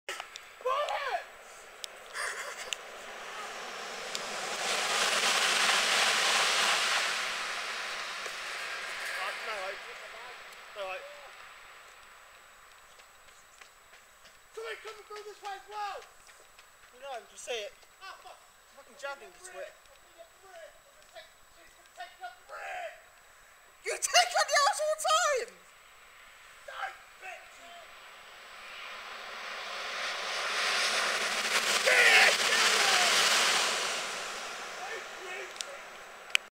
395XXX/395XXX 1F30 + 1J31 @ Rainham (London) 29/06/13
Class 395 Javelins operated by Southeastern are seen passing Rainham (London) working 1F30 12:22 St Pancras International to Faversham and 1J31 11:45 Dover Priory to St Pancras International